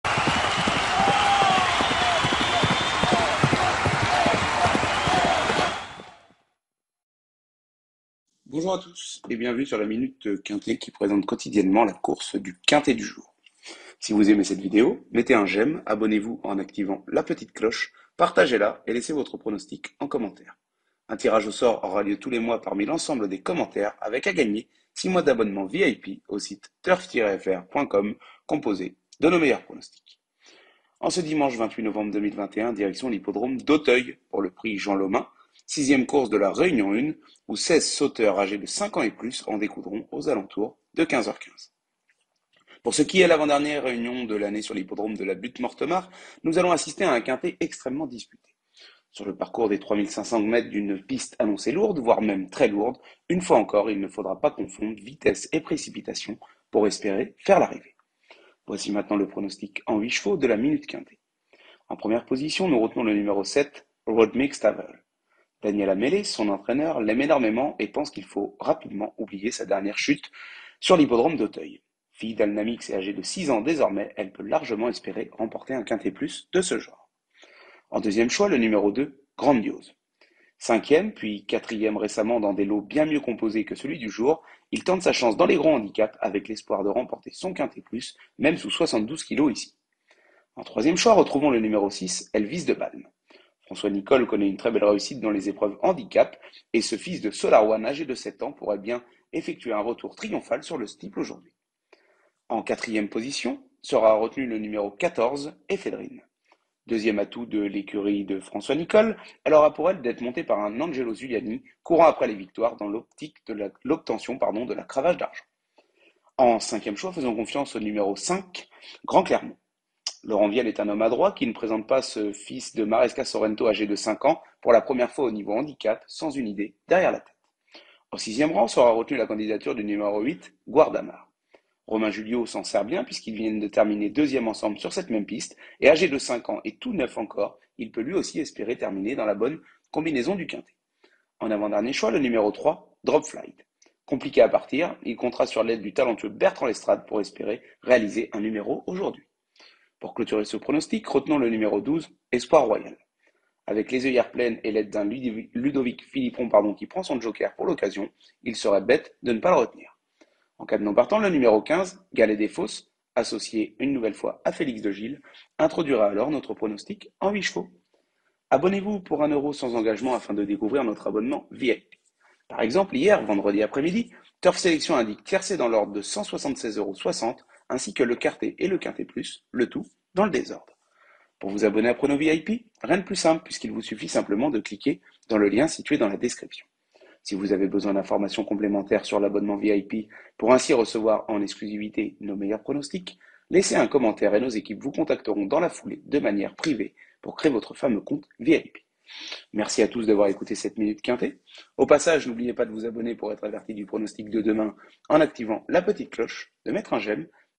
Minute Quinté TURF FR : PRIX JEAN LAUMAIN - Dimanche 28 novembre 2021 - Paris Auteuil 🏇 PMU #260981
🥇 Retrouvez le pronostic exclusif de notre expert Turf Lucius Simenon pour le PRIX JEAN LAUMAIN le Samedi 27 novembre 2021 à l'hippodrome de Paris Auteuil en France.

✅ NOS LOGICIELS TURF :
✔️ Logiciel TURF INFO Professionnel Edition: